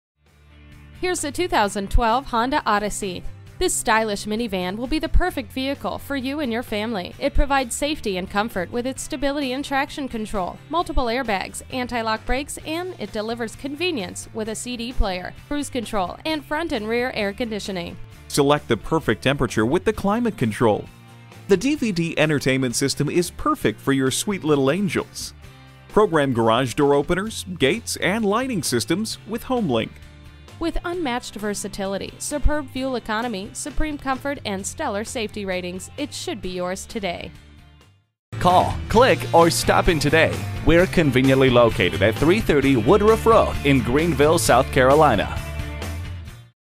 2012 Honda Odyssey Greenville SC Easley, SC 150550A - SOLD
Year: 2012
Make: Honda
Model: Odyssey
Trim: 5 Speed Automatic
Engine: 6 Cylinder
Transmission: 5 Speed Automatic
Color: Alabaster Silver Metallic
Interior: Gray
Mileage: 43333
Stock #: 150550A
VIN: 5FNRL5H68CB030857
HONDA CERTIFIED***7 YEAR/100K WARRANTY***BUY WITH CONFIDENCE!!, DVD!, CLEAN CARFAX! ONE OWNER!, SUNROOF/MOONROOF!, And LEATHER!. Automatic temperature control and Front dual zone A/C. Creampuff! This stunning 2012 Honda Odyssey is not going to disappoint. There you have it, short and sweet! The 2012 Odyssey was named Best Retained Value in the Van category from Edmunds.com. Honda has established itself as a name associated with quality. This Honda Odyssey will get you where you need to go for many years to come. Honda Certified Pre-Owned means you not only get the reassurance of a 12mo/12,000 mile limited warranty, but also up to a 7yr/100k mile powertrain warranty, a 150-point inspection/reconditioning.

Address:
330 Woodruff Rd